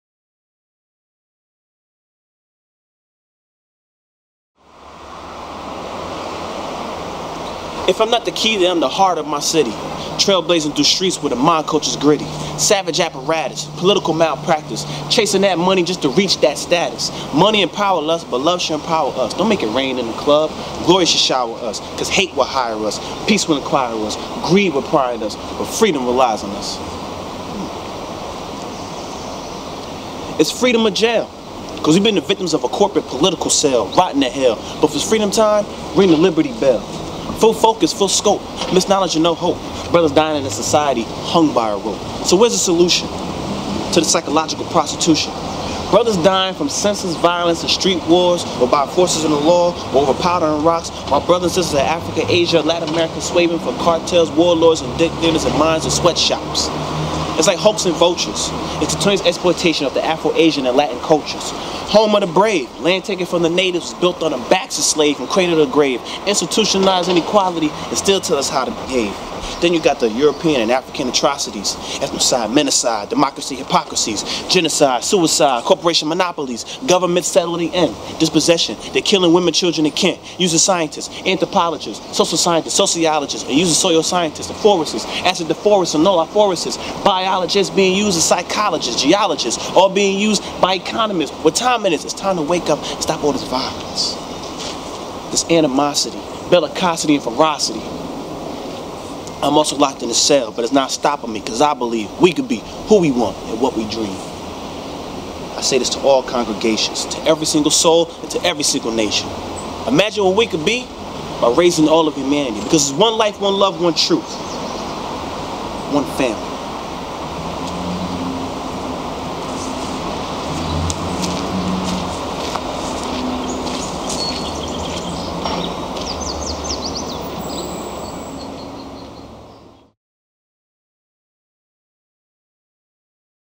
We're The Solution
From The Spoken R.E.B.E.L.s series is a spoken word/poetry piece from our Artician-in-Chief and actor/poet Jaquair Gillette.

Produced by Sean English, Akiba Rhodes, Jaquair Gillette

Special Contributions by Zhenyu Lin  &  Jack Hsu

Connect With The R.E.B.E.L. Experience